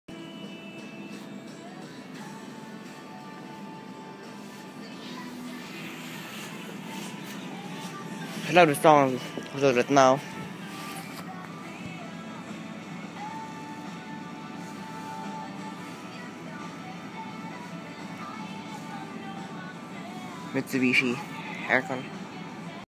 12" oscillating ceiling fans at a convenience store
It looks like they were made as wall fans and just converted to ceiling fans.